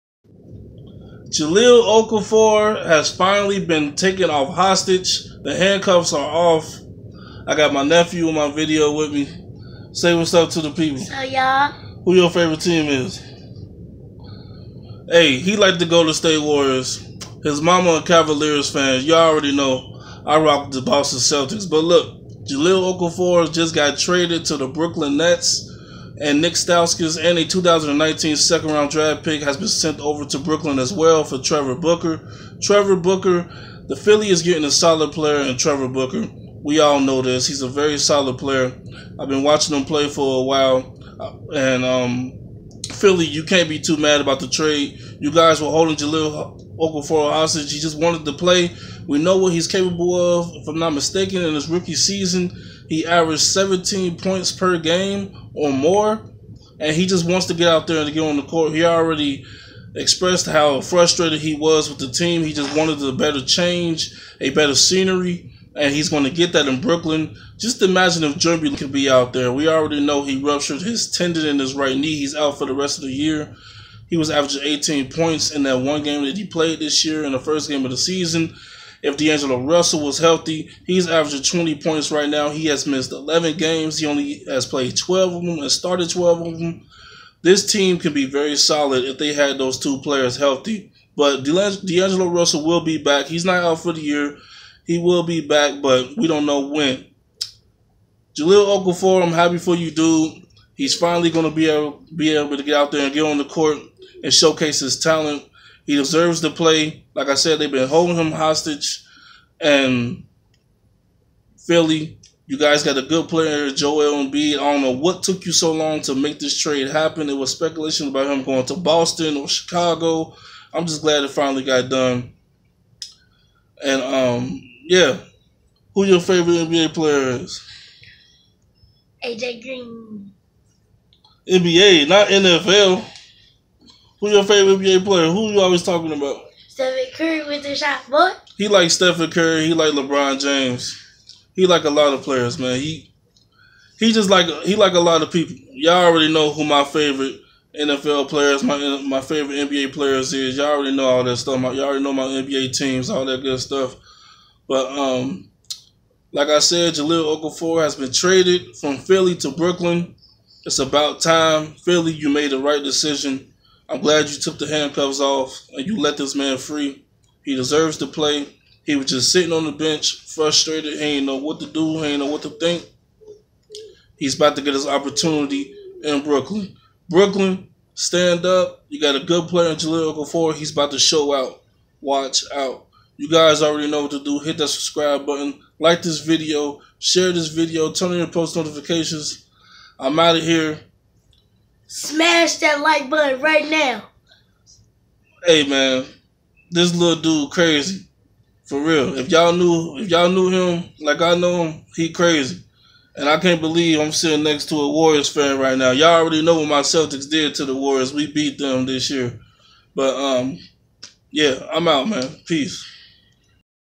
Jahlil Okafor is FREEEEE! Traded To Brooklyn Nets For Trevor Booker
Here to provide you with great analysis and sports talk several days of the week.